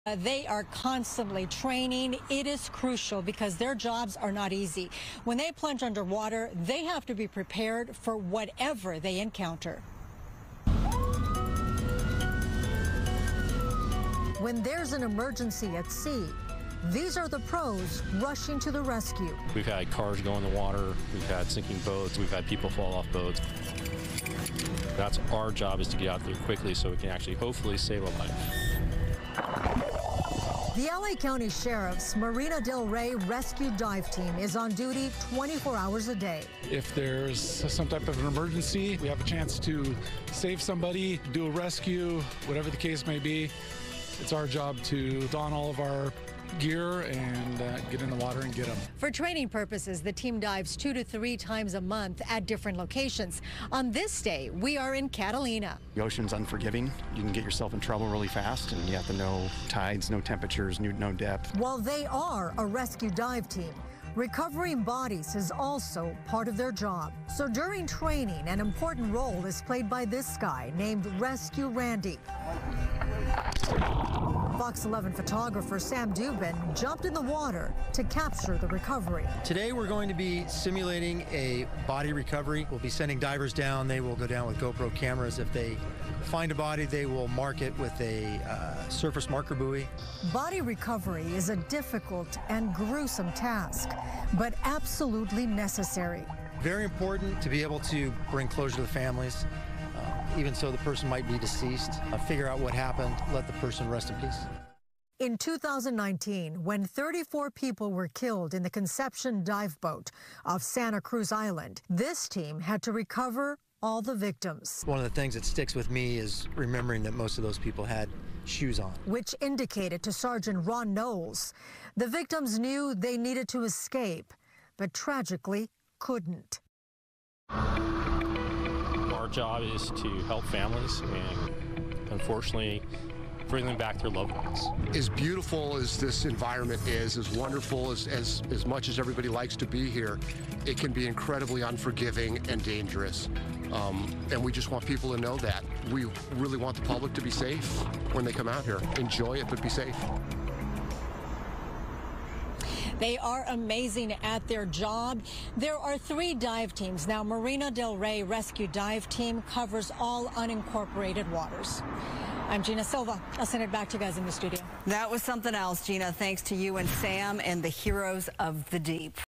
Guardians of the deep: Meet LA County Sheriff's rescue dive team
Their mission is to bring missing loved ones homes -- even if the outcomes are tragic.